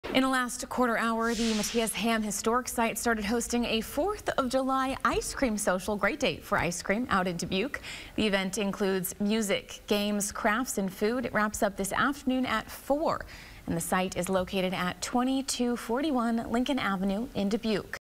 Mathias Ham Historic Site hosts 4th of July Ice Cream Social
The event includes music, games, crafts, and food.  It wraps up Tuesday at 4:00 pm at the site.